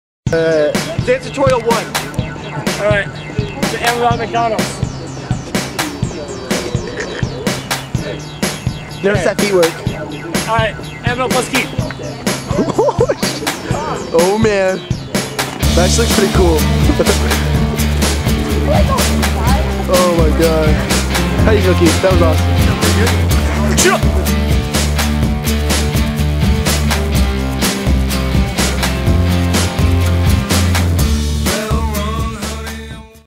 Adventures in Tracy Chris Dance tutorial
Chris is gonna teach the world how to dance